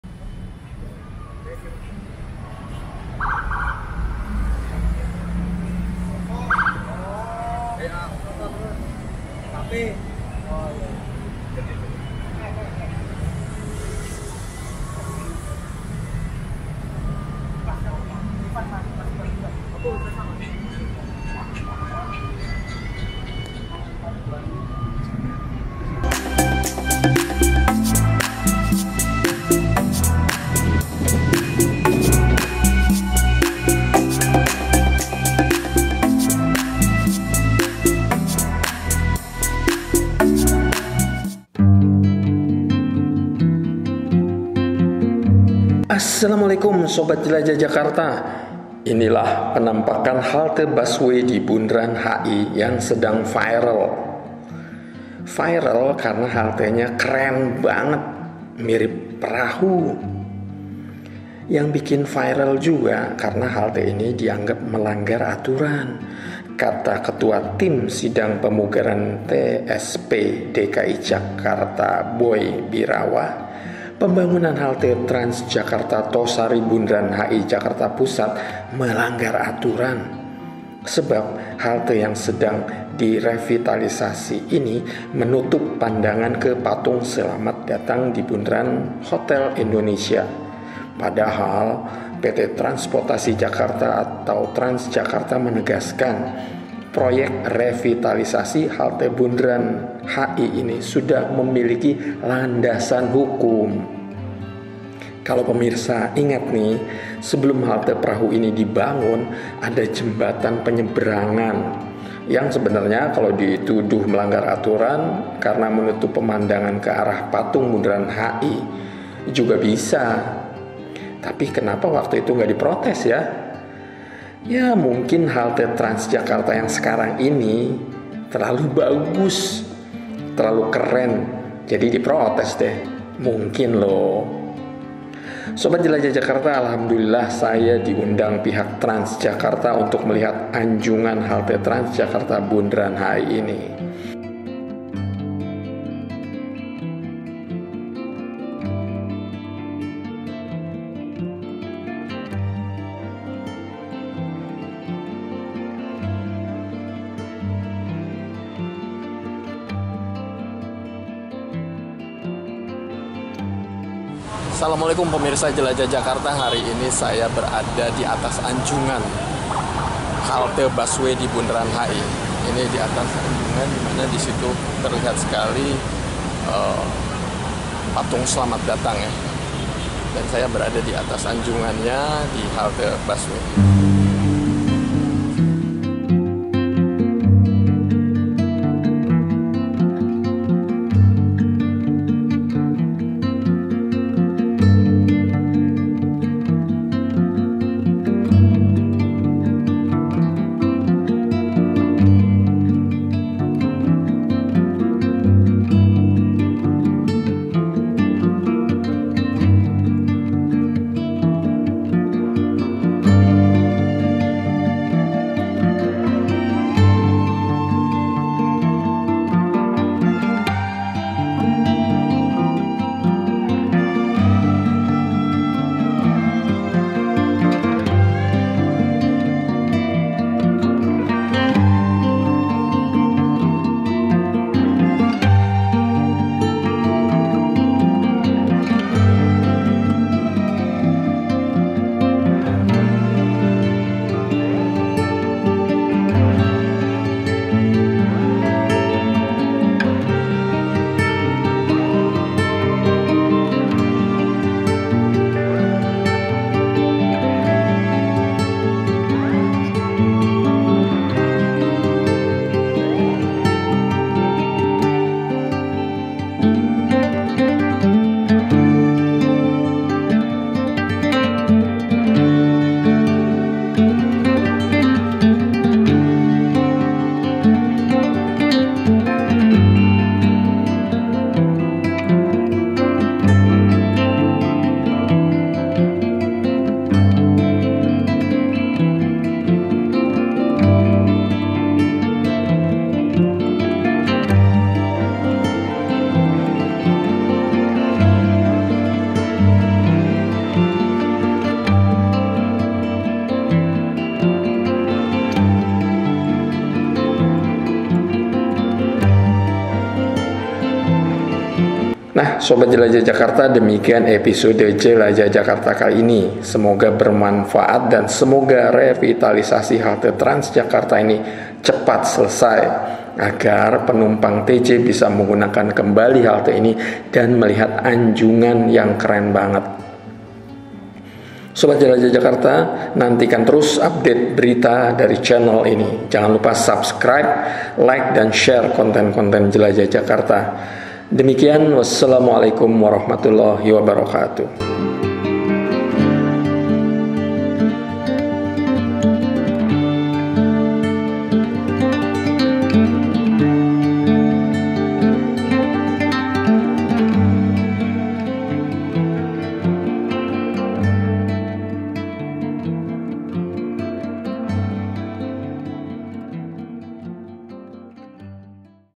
Penampakan "Halte Perahu" TransJakarta - Bakal Jadi Ikon Baru Jakarta, nih! KEREN BANGET!
Halte TransJakarta masih terus direvitalisasi alias belum 100% rampung. Namun penampakan haltenya sudah terlihat keren banget. Halte ini bakal jadi ikon baru Jakarta. Beruntunglah mereka yang pakai TransJakarta kelak bisa berada di anjungan "perahu" halte yang bisa menjadi objek swafoto dengan background Patung Selamat Datang.